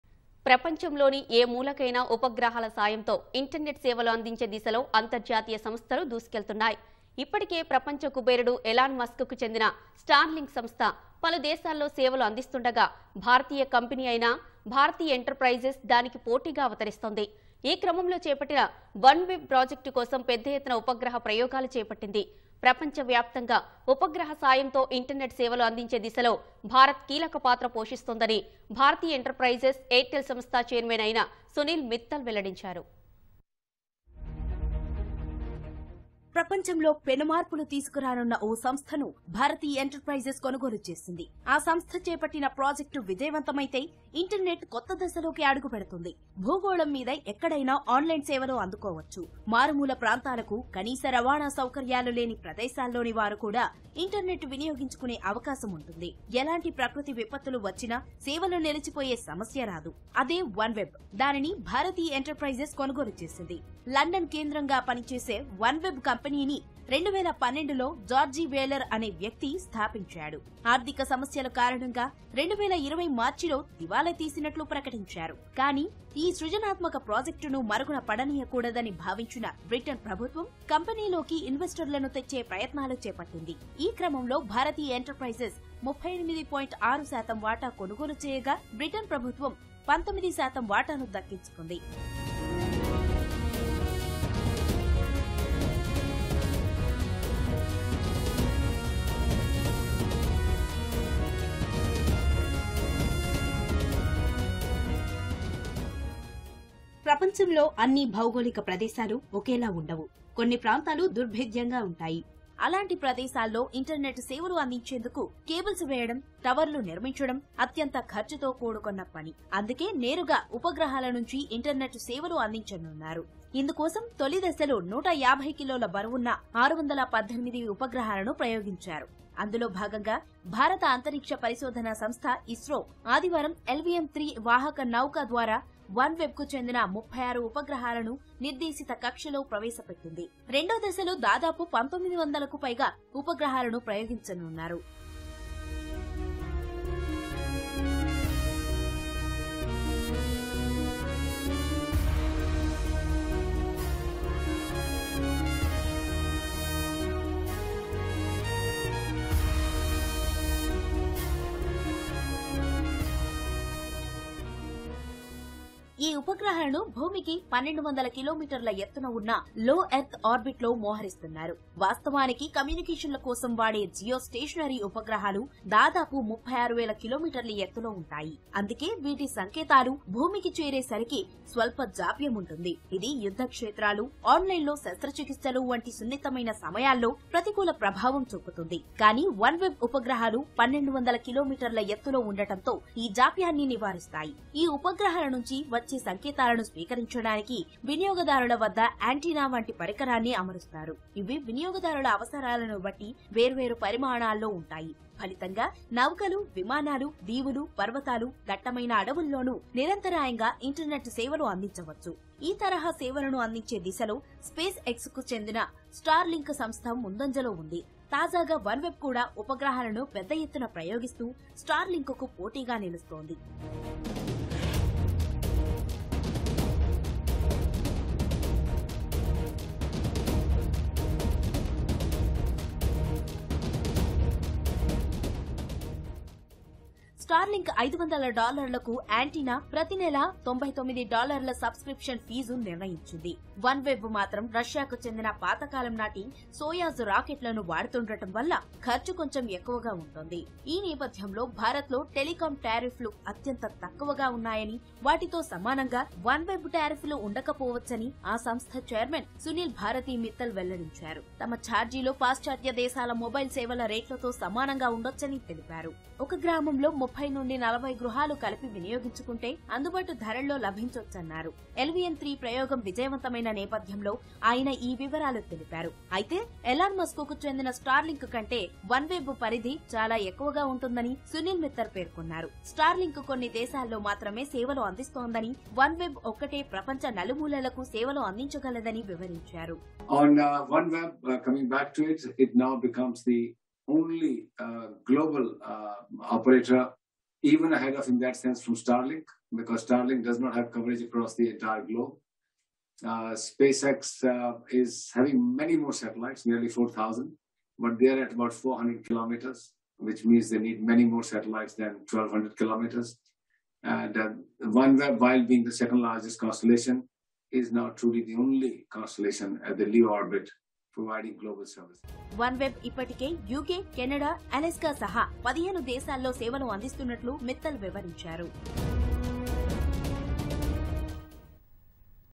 One Web Project | Ready to Offers Broadband Services | Global Wide | Airtel Chairman Sunil Mittal
ప్రపంచంలోని ఏ మూలకైనా ఉపగ్రహాల సాయంతో... ఇంటర్నెట్‌ సేవలు అందించే దిశలో అంతర్జాతీయ సంస్థలు దూసుకెళుతున్నాయి.ఇప్పటికే ప్రపంచ కుబేరుడు ఎలాన్‌ మస్క్‌కు చెందిన స్టార్‌లింక్‌ సంస్థ. పలు దేశాల్లో సేవలు అందిస్తుండగా. భారతీయ కంపెనీ అయిన భారతీ ఎంటర్‌ ప్రైజెస్‌‌ దానికి పోటీగా అవతరిస్తోంది. ఈ క్రమంలో చేపట్టిన వన్‌వెబ్‌ ప్రాజెక్టు కోసం. పెద్దఎత్తున ఉపగ్రహ ప్రయోగాలు చేపట్టింది. ప్రపంచవ్యాప్తంగా. ఉపగ్రహ సాయంతో ఇంటర్నెట్‌ సేవలు
అందించే దిశలో భారత్‌ కీలకపాత్ర పోషిస్తుందని. భారతీ ఎంటర్‌ప్రైజెస్‌, ఎయిర్‌టెల్‌ సంస్థ ఛైర్మన్‌ అయిన సునీల్‌ మిత్తల్‌ వెల్లడించారు.